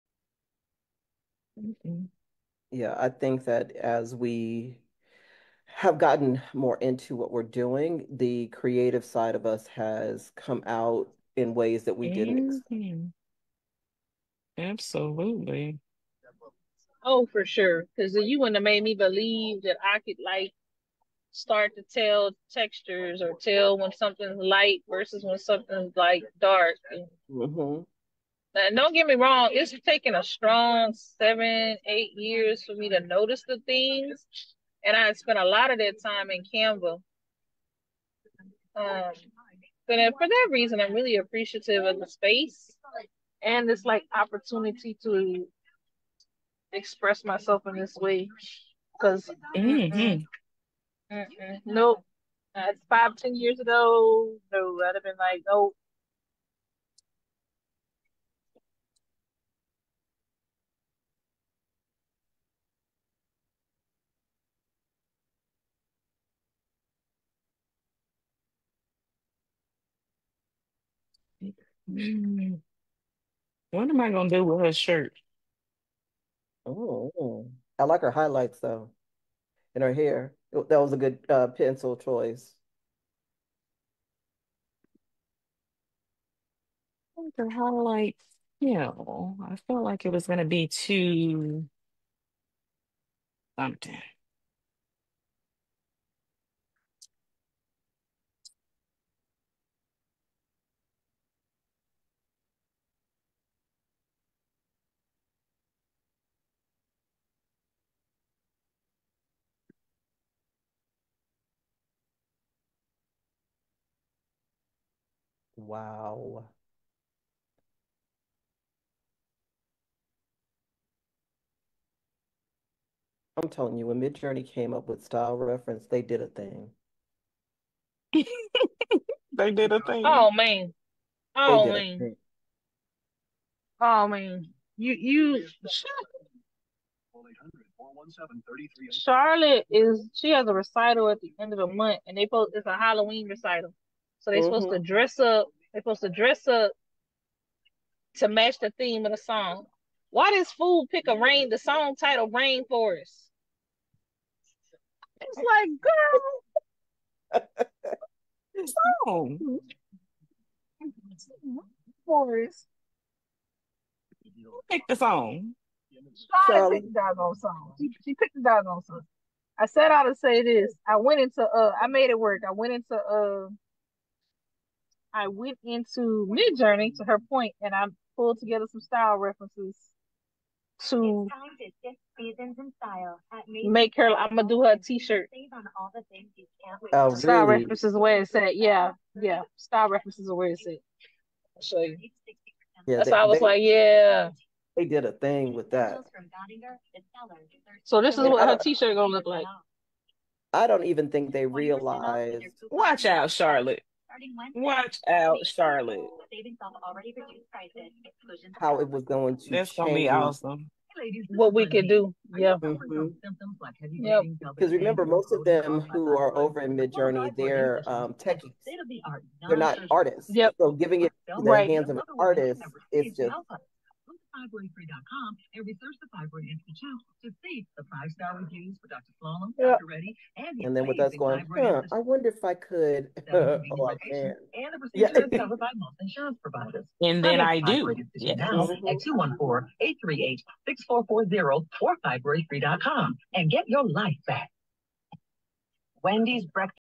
Unlocking Creativity: Textures and Style in Midjourney
Explore how we can express ourselves creatively through textures and styling, using Midjourney as a powerful tool. Join us as we share insights on transforming design concepts, with a fun Halloween recital project for Charlotte!